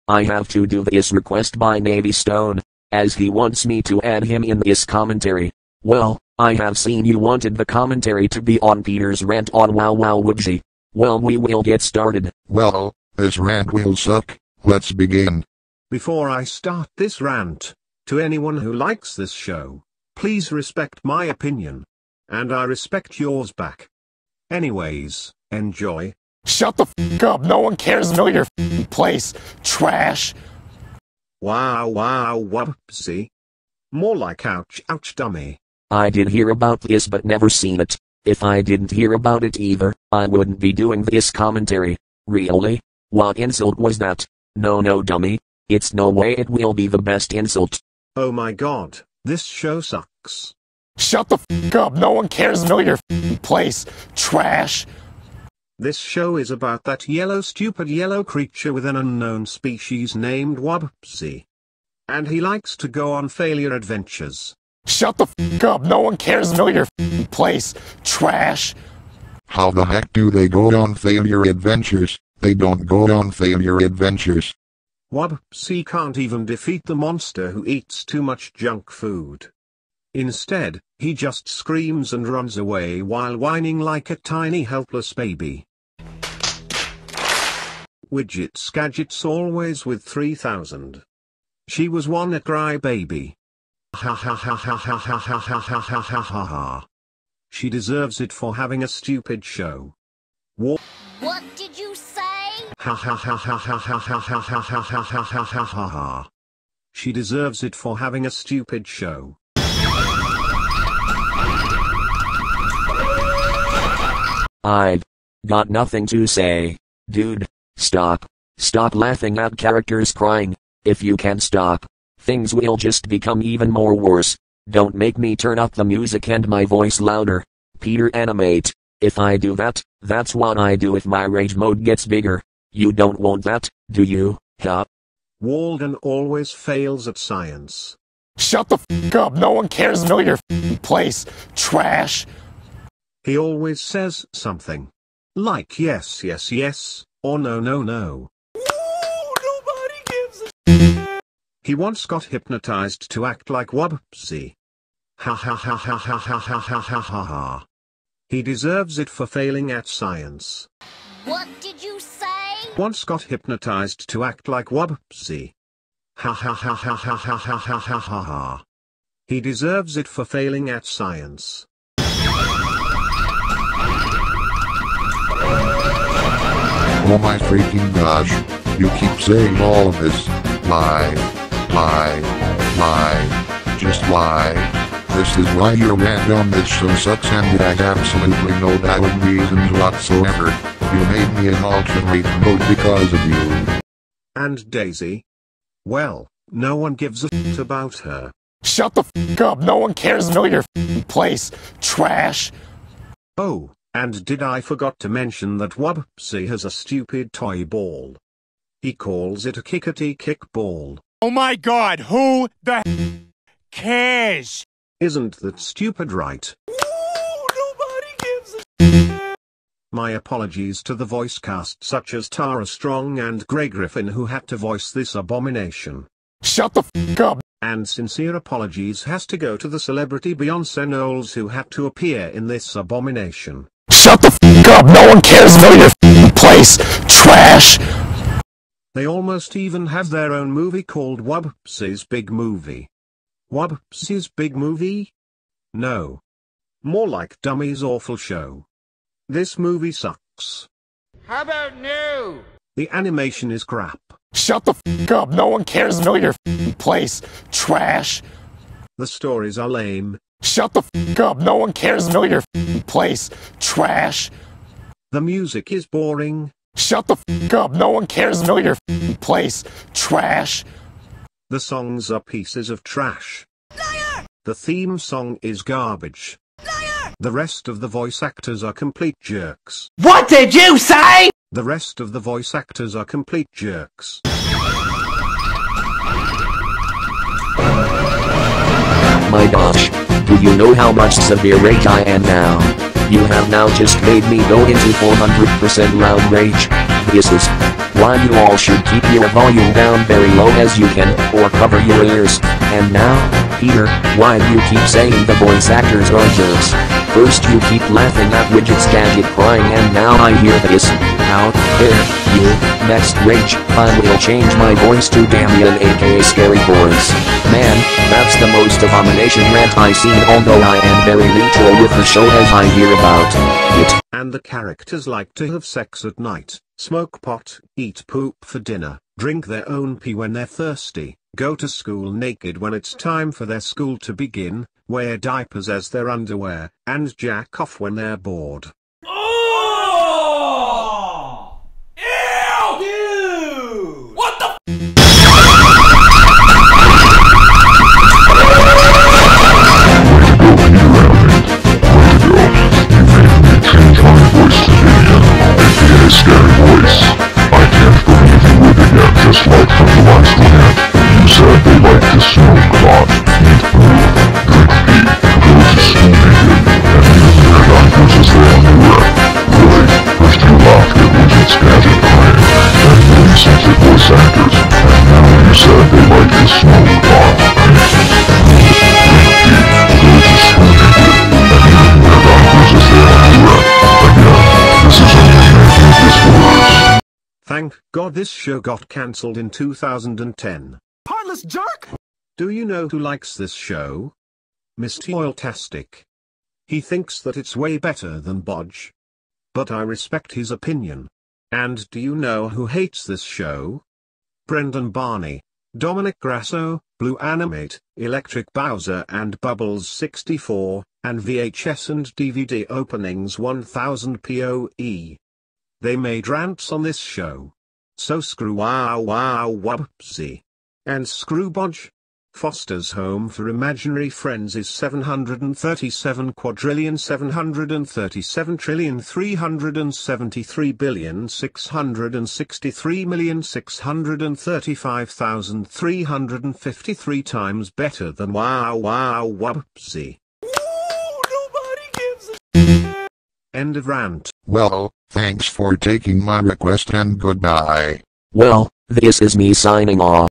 Kasey Commentaries S2 E5: Peter's Rant on Wow Wow Wubbzy Ft. Special Guest, Navystone
This was a request by Navystone

PeterAnimate is always treating shows he hates like they're worse than COVID-19, in reality, COVID-19 is by far way worse. And COVID-19 doesn't have 1 thing to do with this rant by PeterAnimate